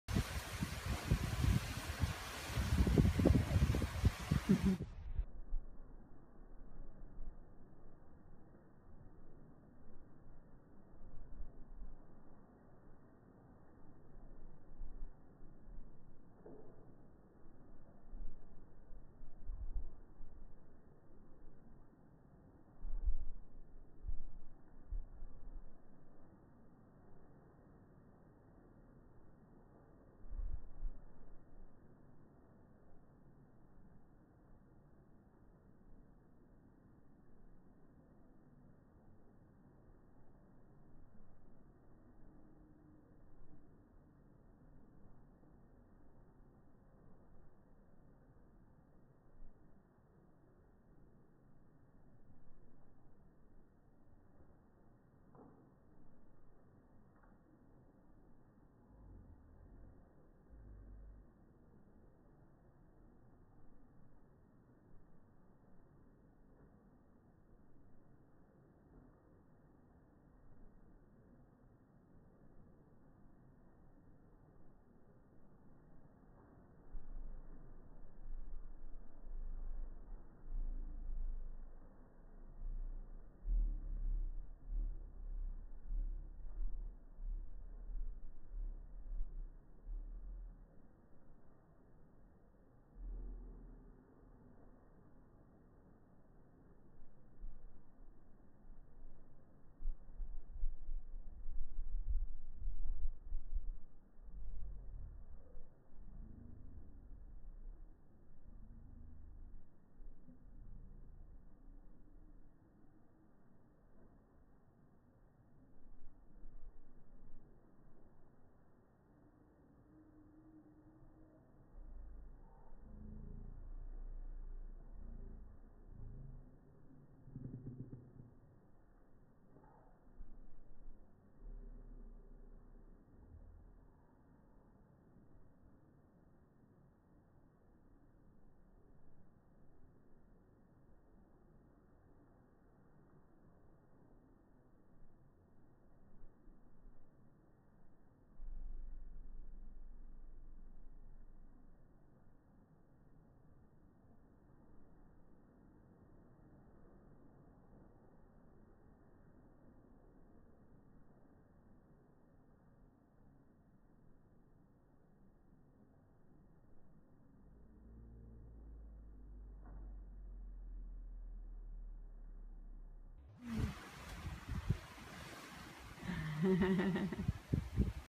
Slow motion swinging in the wrap conversion swing -11th Oct
Made by me, from a Girasol Zenith Azul baby wrap.